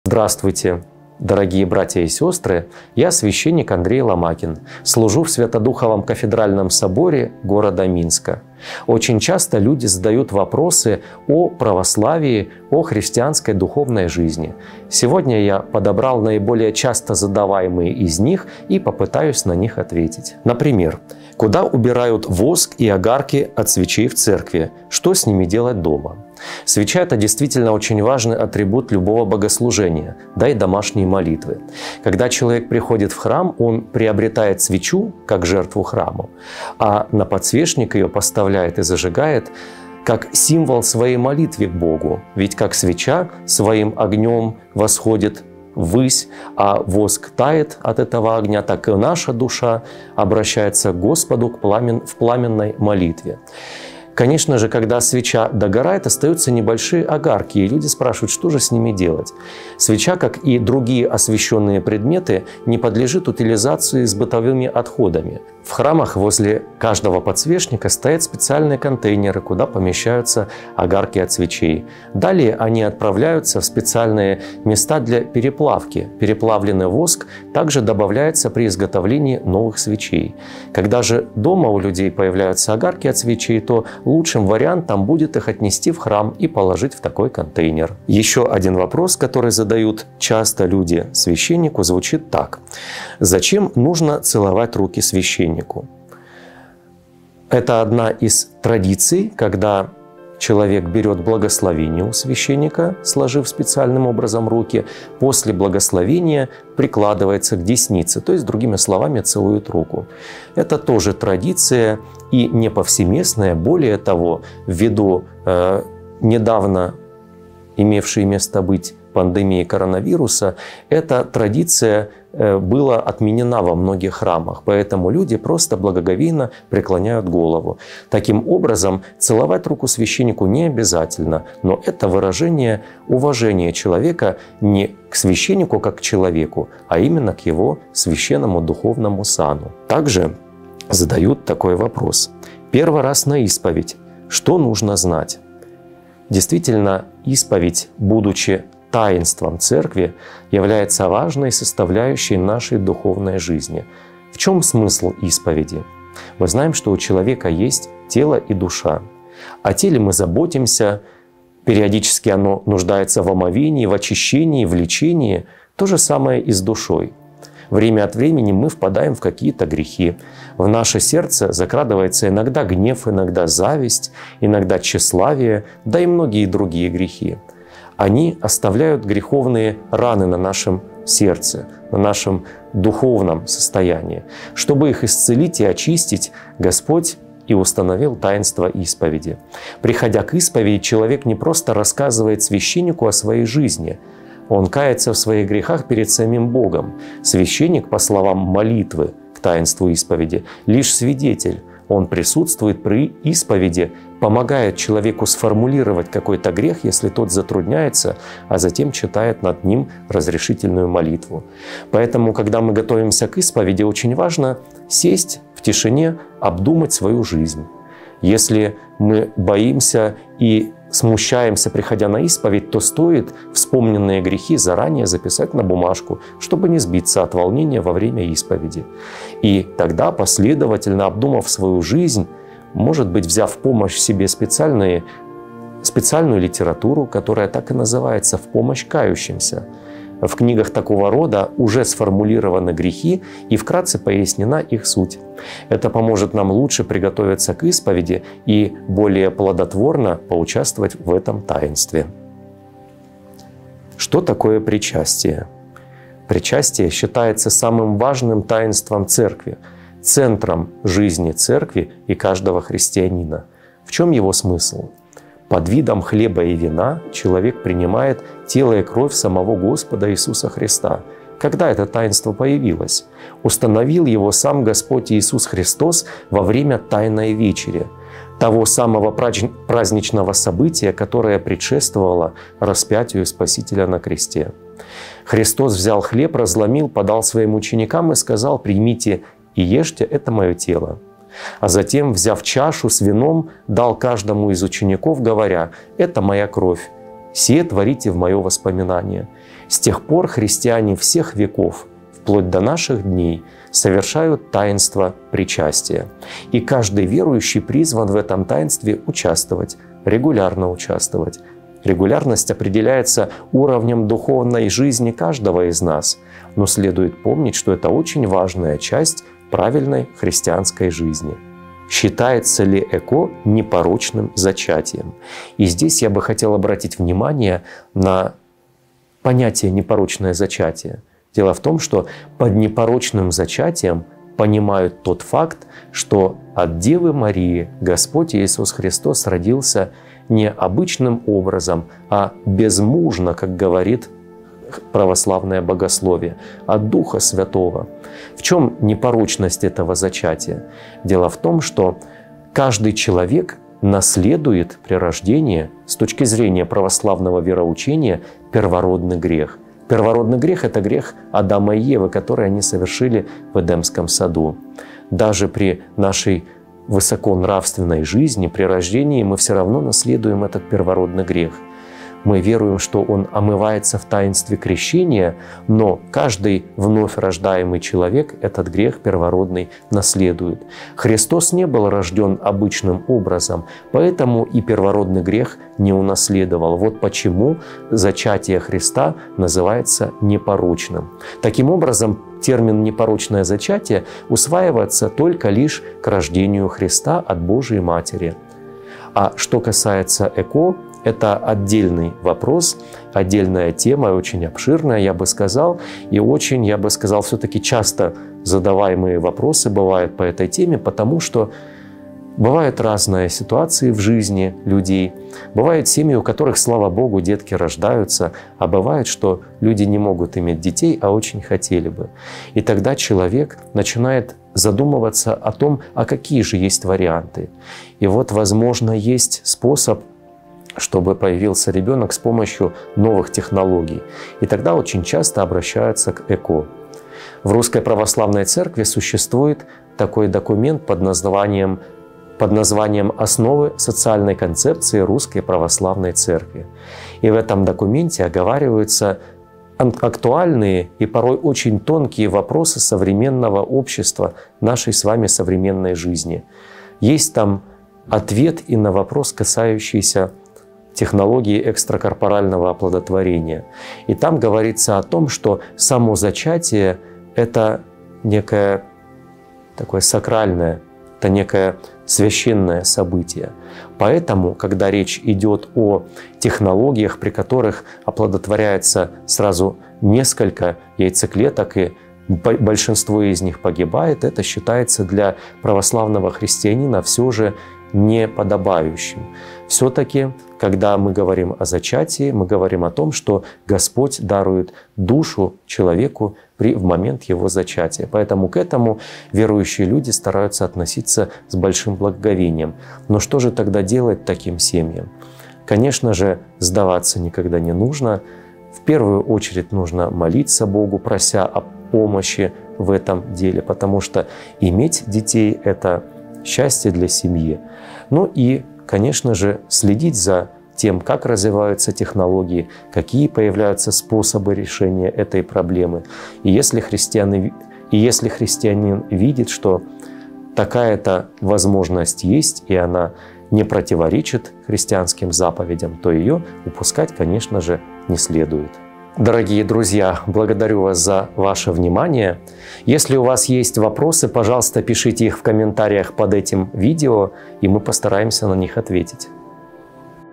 ТОП-5 вопросов священнику
Иерей Андрей Ломакин, клирик Свято-Духова кафедрального собора города Минска отвечает на самые задаваемые вопросы о православии и христианско-духовной жизни.

00:21 — Куда убирают воск и огарки от свечей в церкви?  Что с ними делать дома?
01:28 — Зачем нужно целовать руки священнику?
02:20 — Первый раз на исповедь. Что нужно знать?
04:15 — Что такое причастие?
05:21 — Считается ли ЭКО непорочным зачатием?